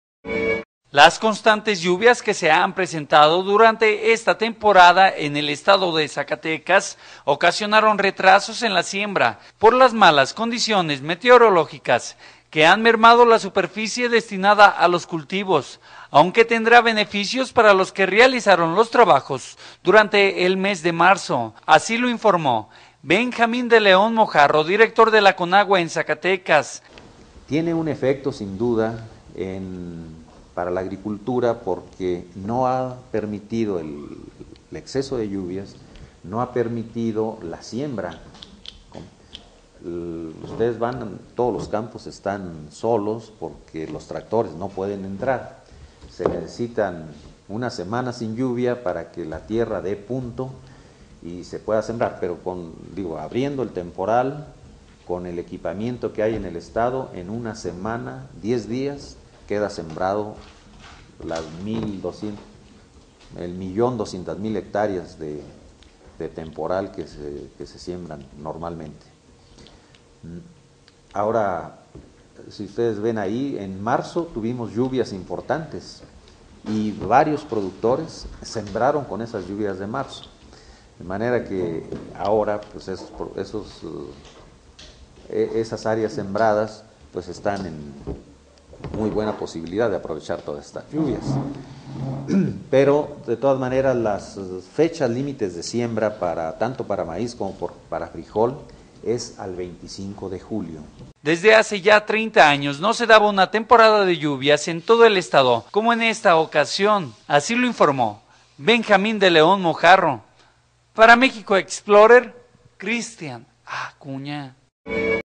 Lluvias recientes ocasionan retrasos en la siembra para algunos agricultores zacatecanos
Las constantes lluvias que se han presentado durante esta temporada en el estado de Zacatecas, ocasionaron retrasos en la siembra por las malas condiciones meteorológicas que han mermado la superficie destinada a los cultivos, aunque tendrá beneficios para los que realizaron los trabajos durante el mes de marzo  así lo informó  Benjamín de León Mojarro, director de Conagua en Zacatecas